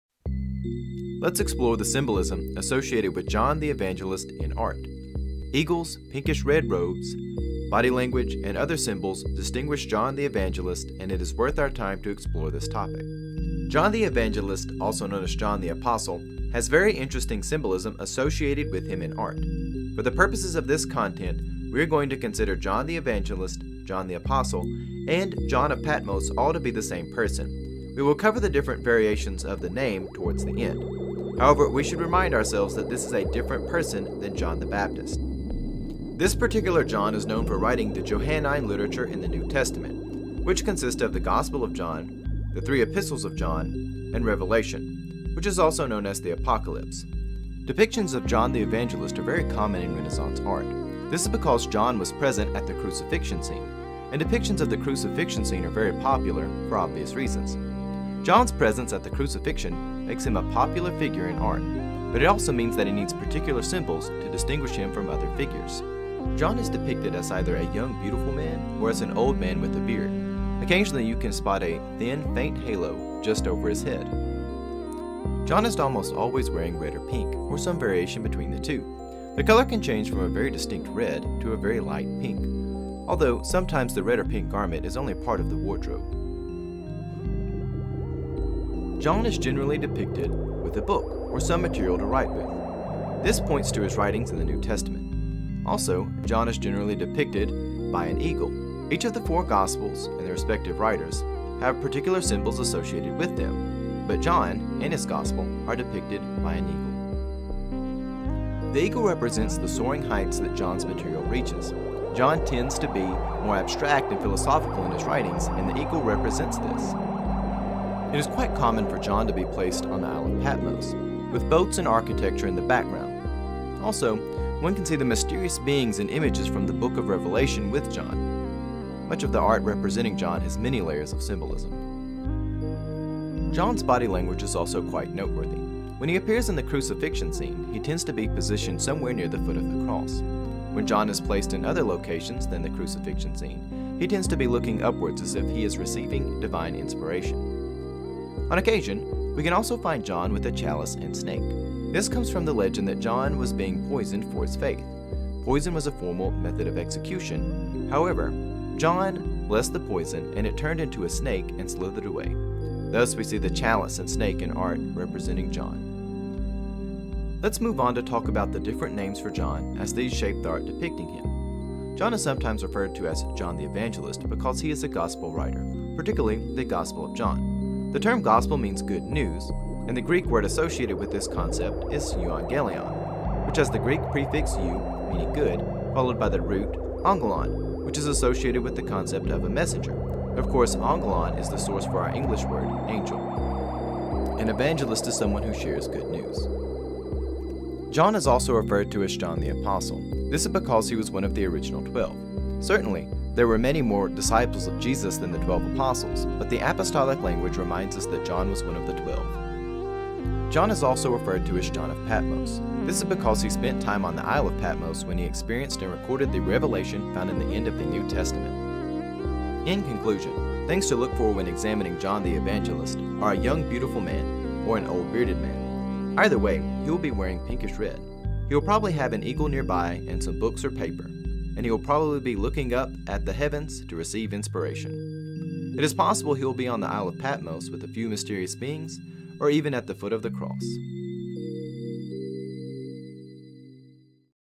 The Hidden Symbols in Art Depicting John the Evangelist (Apostle John)
Art Depicting John the Evangelist (and also John the Apostle, John of Patmos) is filled with symbols, in this overview we examine their origins